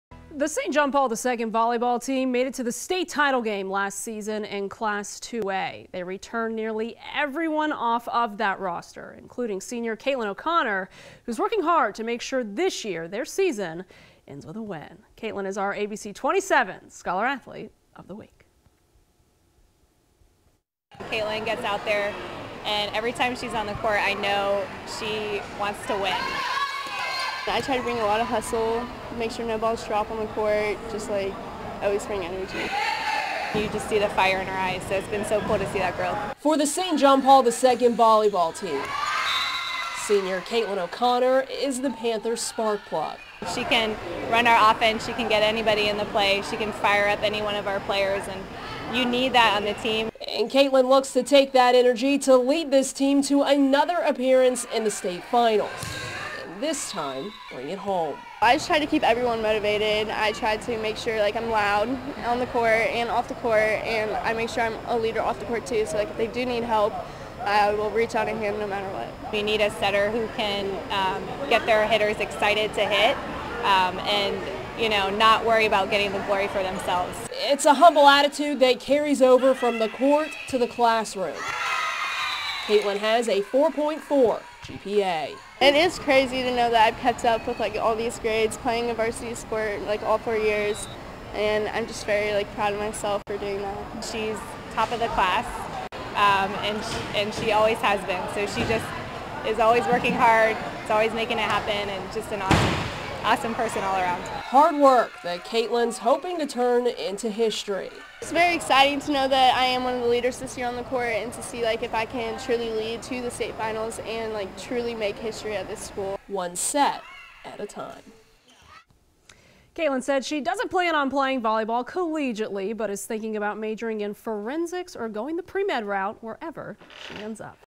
ABC 27 Scholar Athlete of the Week: St. John Paul II's Caitlin O'Connor
This week's Scholar Athlete of the Week Presented by D1 Training Tallahassee is St. John Paul II's Caitlin O'Connor.